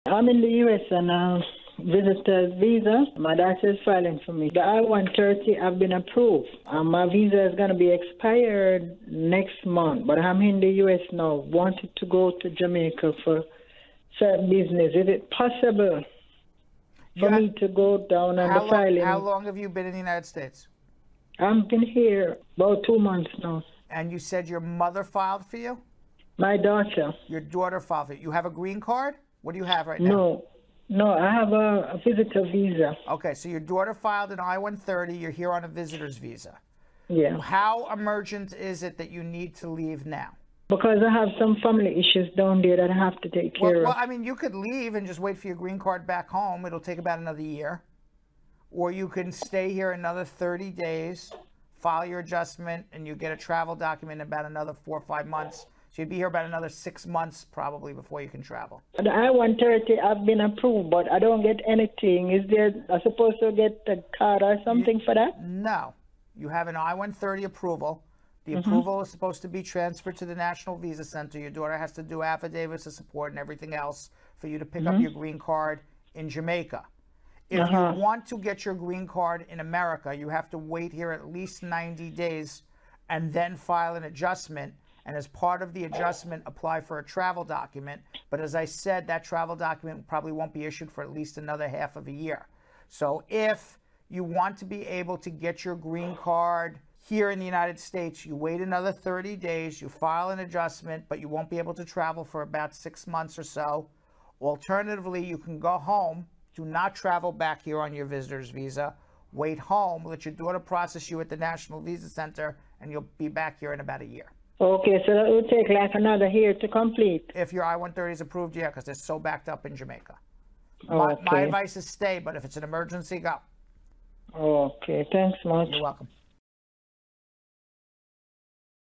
Can I Travel Back To My Home Country After My I-130 Has Been Approved?
Super Lawyer Brad Bernstein assists a caller who is in the United States on a visitor visa. Her daughter filed for her I-130, and it got approved. She wants to go back to her home country of Jamaica to handle some family business, but she is inquiring whether taking a trip back home is going to prevent her from receiving her green card.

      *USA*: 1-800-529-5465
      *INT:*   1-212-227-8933

🏢 VISIT
     Law Offices of Spar Bernstein
     225 Broadway, 5th FL
    New York, New York 10007